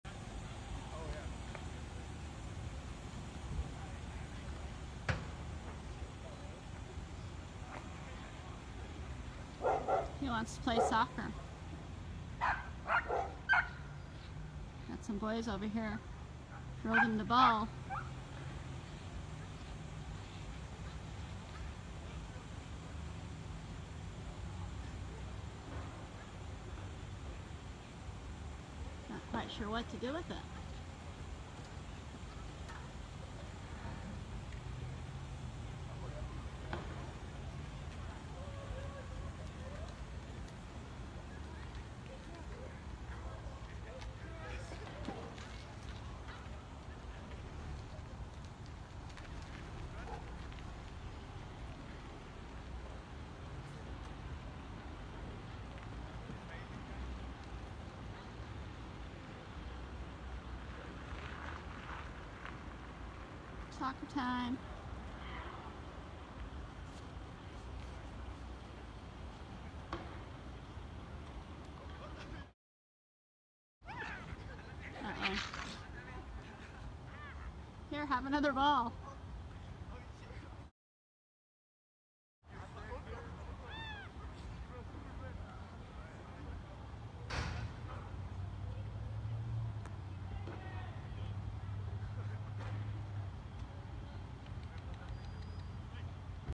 Elk 'plays' soccer on a field in Payson
An elk plays soccer on a field in Payson. Juline Webb Curtis captured this compelling video while on her way home from work.